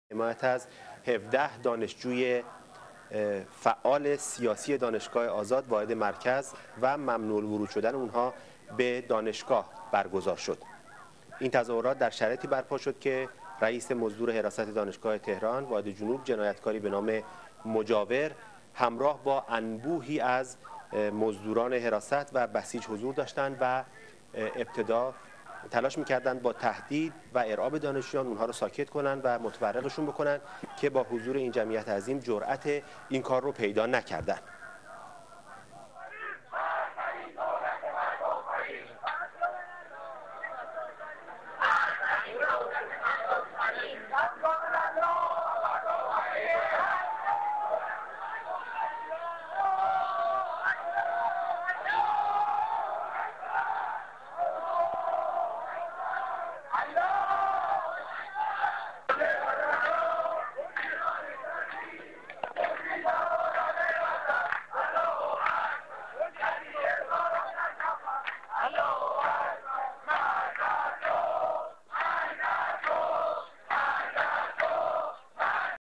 Tehran azad south
demo in Free University - south of Tehran, 27 october
manif à l'université libre -sud de Téhéran, 27 octobre